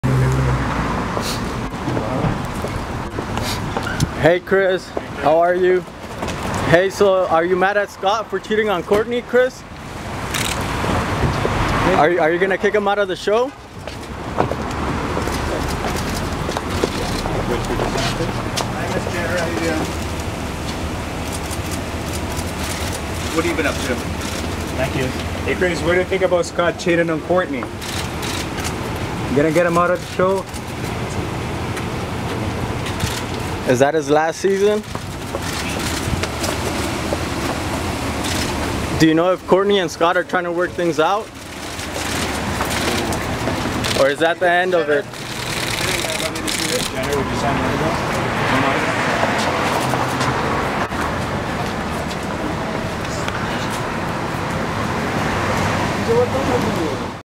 PREMIUM EXCLUSIVE - Kris And Kylie Jenner Get Lunch, Upset When Asked About Firing Scott Disick
Tuesday, July 7, 2015 - Kris Jenner looks forlorn as she gets lunch with Kylie at Craig's and is asked if she will fire Scott Disick from "Keeping Up With the Kardashian's." Kris shows off her trim figure in a figure hugging black dress and daughter Kylie bares her midriff in a black top and long white coat.